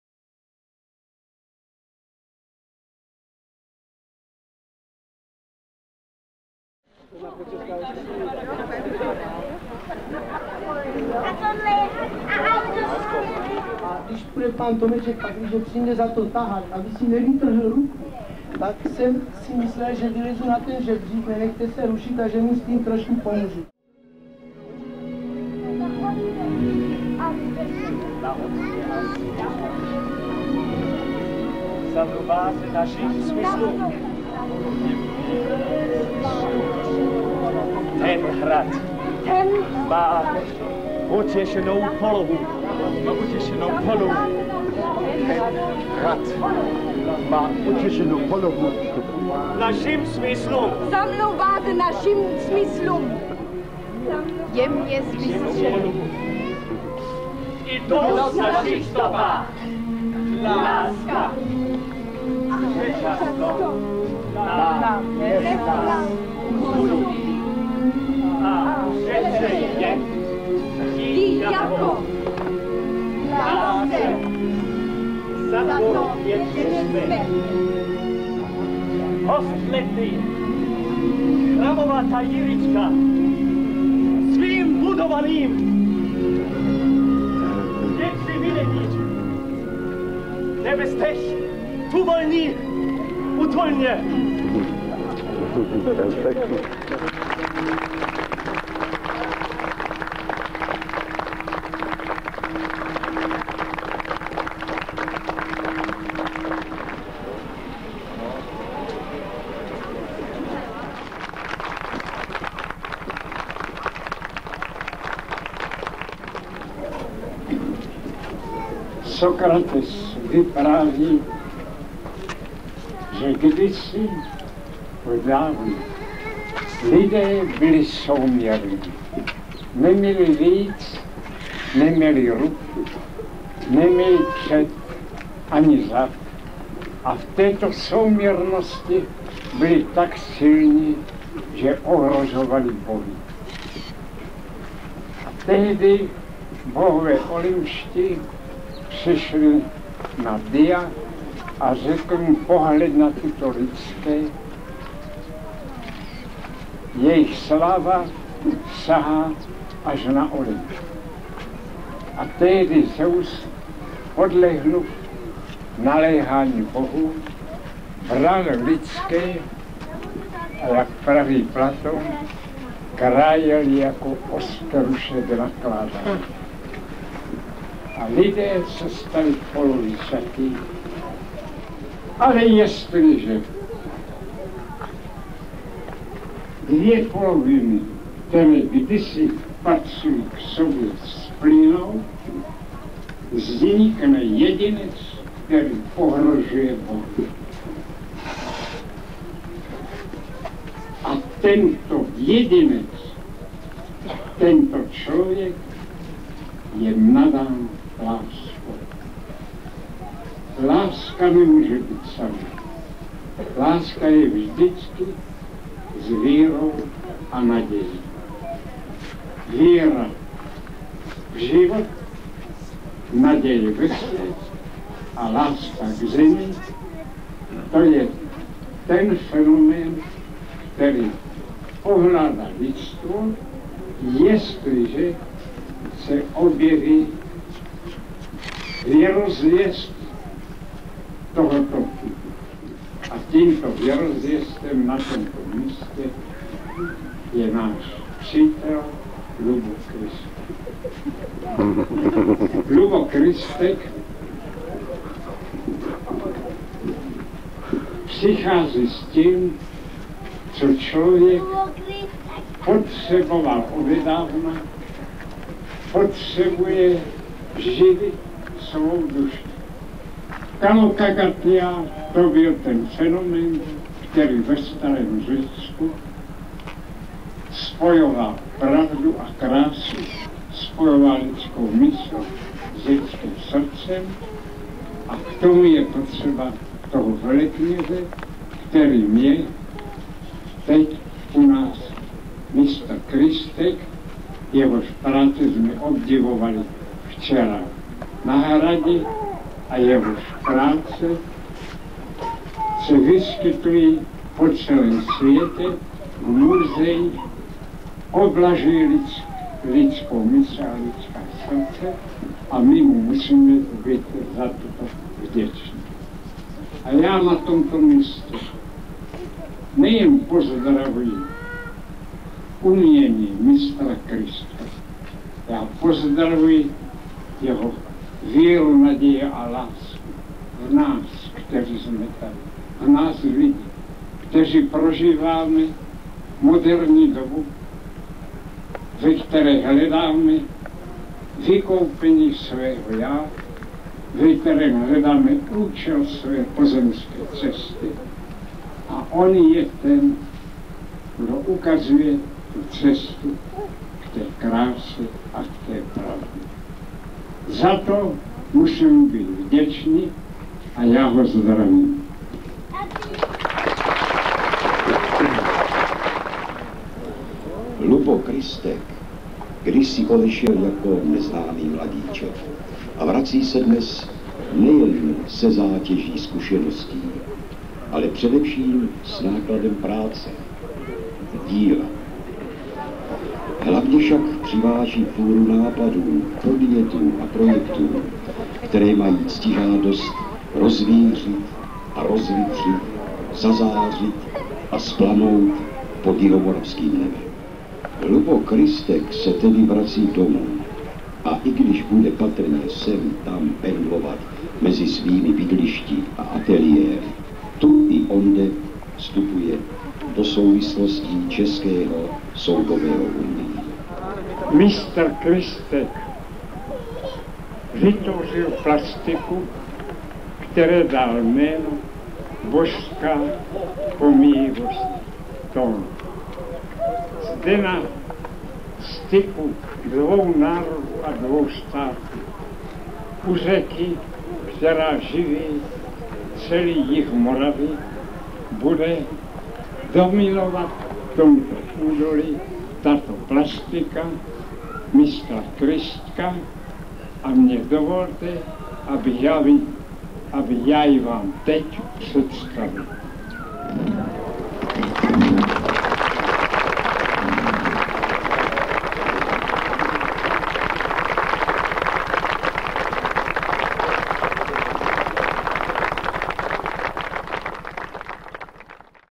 Lubo Kristek 1994 - revelation of the symbol of the artist - Podhradí nad Dyjí (CZ)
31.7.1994
Revelation of the symbol of the artist "Divine Transcience of a Tone"
Enthüllung des Symbol des Künstler "Die göttliche Vergänglichkeit des Tones"
Odhalení symbolu umělce "Božská pomíjivost tónu"